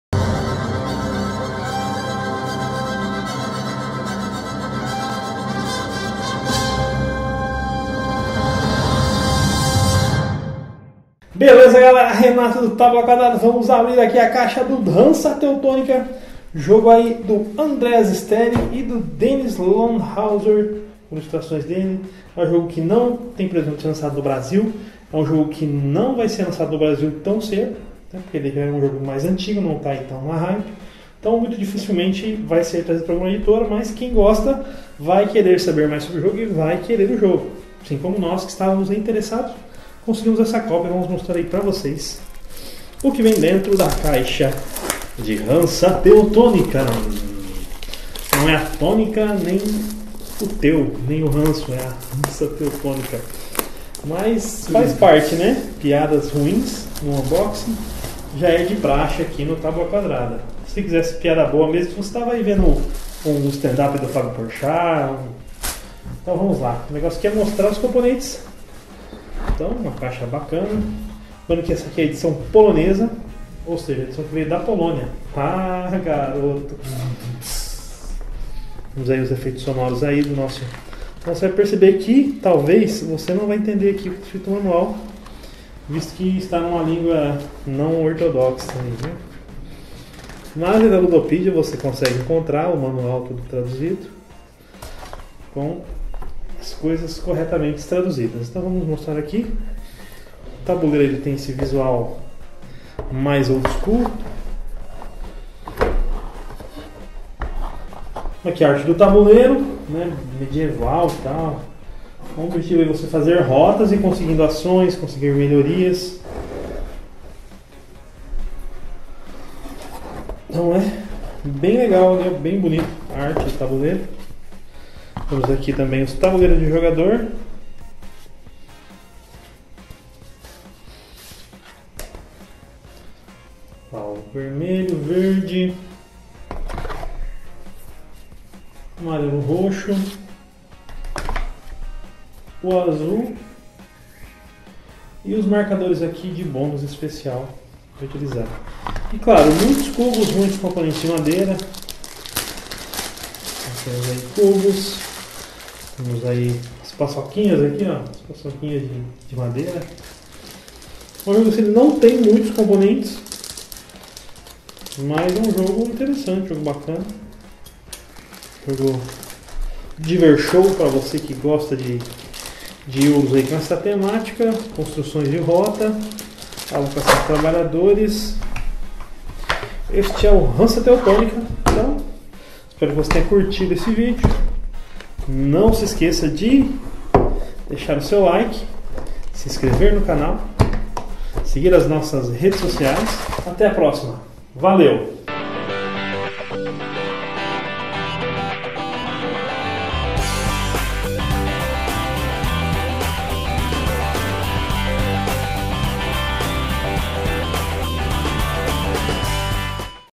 Unboxing 101 - Hansa Teutonica
Hansa Teutonica é um clássico dos jogos de tabuleiro que trouxe novidades utilizadas até hoje em grandes jogos. Terra Mystica, Clans of Caledonia, Scythe e muitos outros são frutos do que foi criado por Andreas Steding no ano de 2009. Dá uma olhada no nosso review escrito sobre esse jogão.

Renato J. Lopes abre a caixa de Hansa Teutonica e mostra os componentes da última edição lançada na Europa. Um jogo que não depende de texto e têm o manual traduzido na Ludopedia. Confira no vídeo.